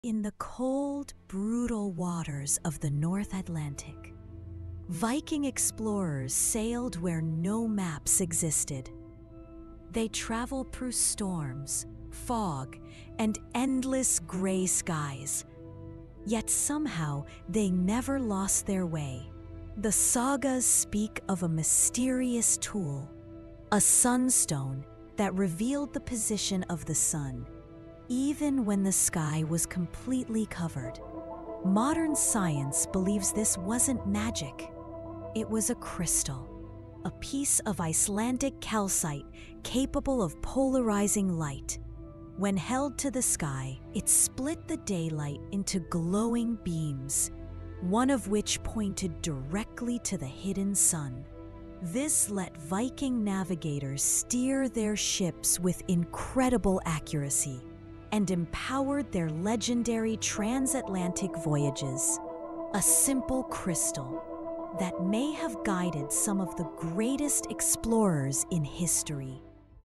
How Vikings Navigated the Ocean Without Sunlight
Viking legends tell of a mysterious “sunstone” that could locate the hidden sun even on stormy, overcast days.
Modern science suggests this was a calcite crystal capable of polarizing light—allowing navigators to steer across the North Atlantic with stunning accuracy.
This cinematic sequence brings Viking sunstone navigation to life.